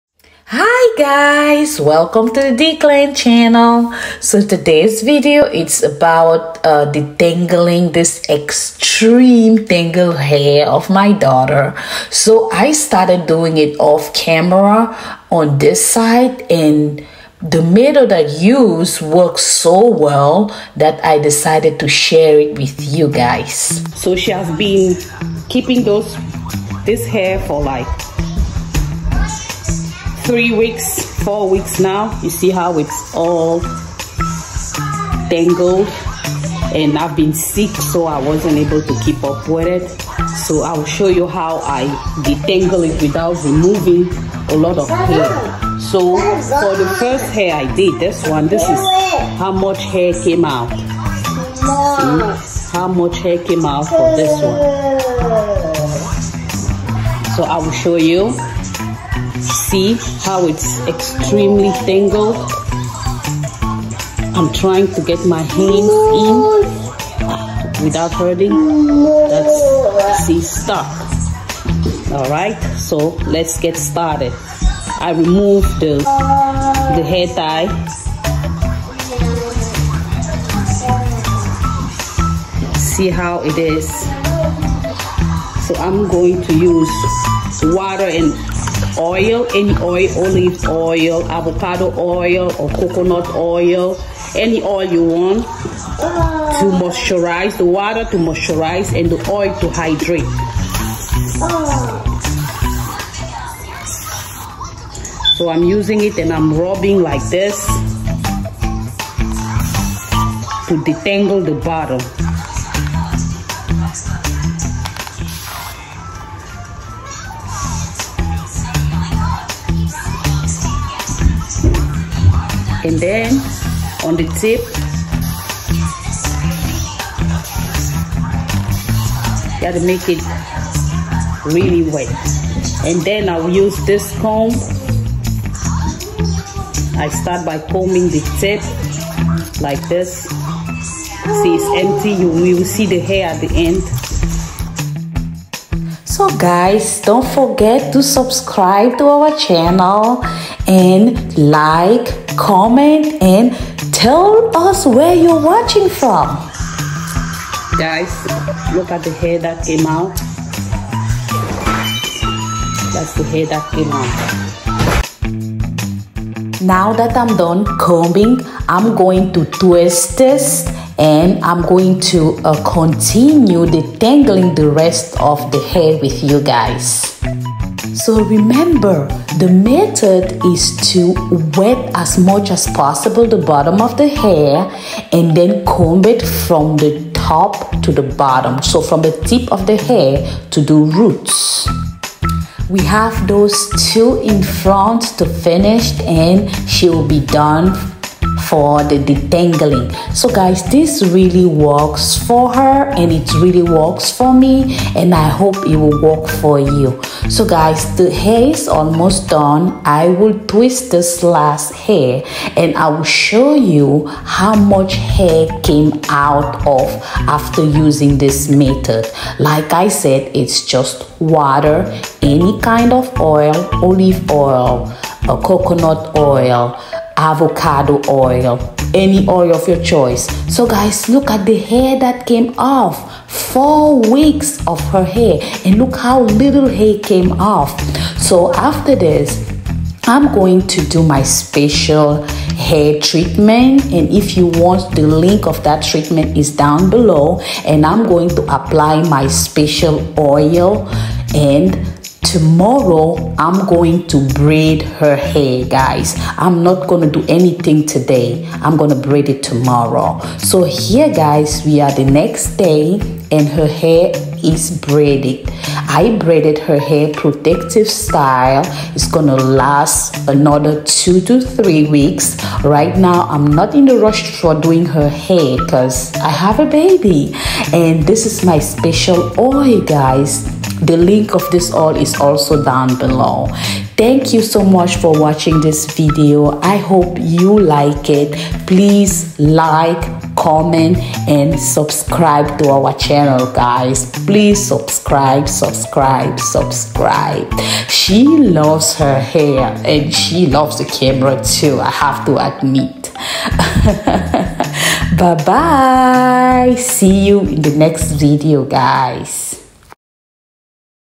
How to Detangle toddler extremely matted hair with only this ingredient/works with any type of hair
This video will help you detangle any type of hair without breakage and not much hair lost in the process. You only need water any any oil of your choice plus a spray bottle.